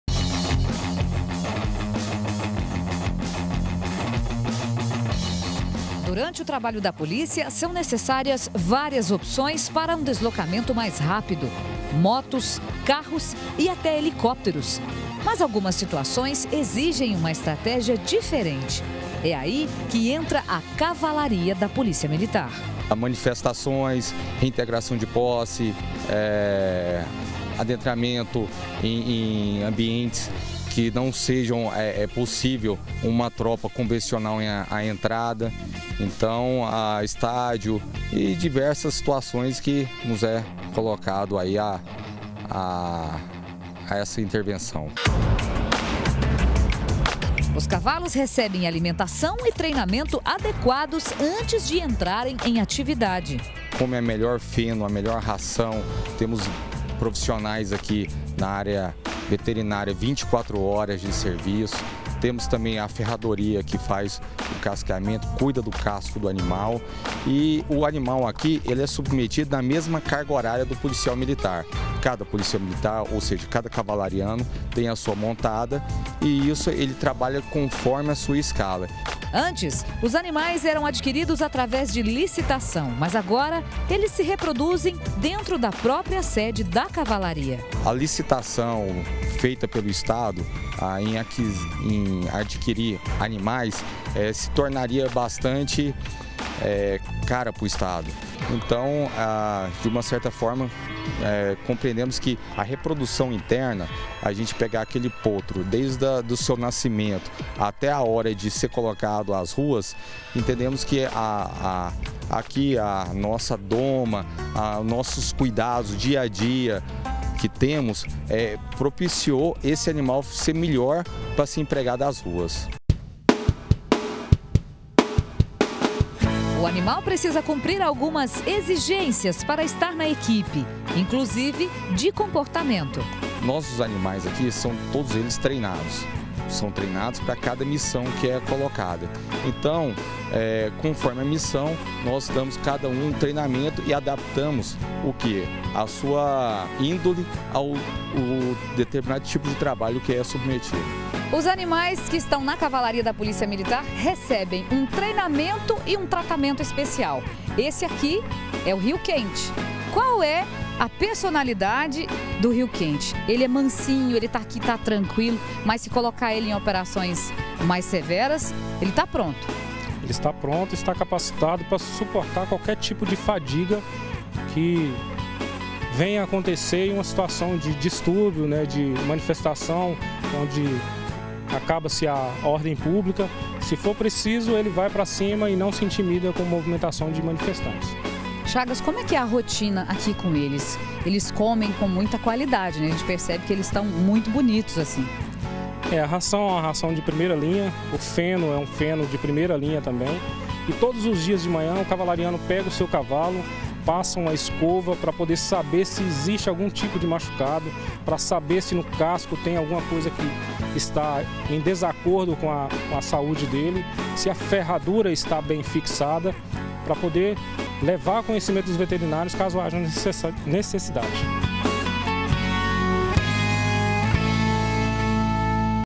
DR - O trabalho da cavalaria da polícia militar - 25-06-2018
Agora, vamos mostrar um trabalho muito bacana da polícia militar. A cavalaria, que ajuda em situações difíceis, como por exemplo, em dias de jogos no Serra Dourada ou manifestações. E pra atuar nessas situações, os cavalos recebem uma preparação especial.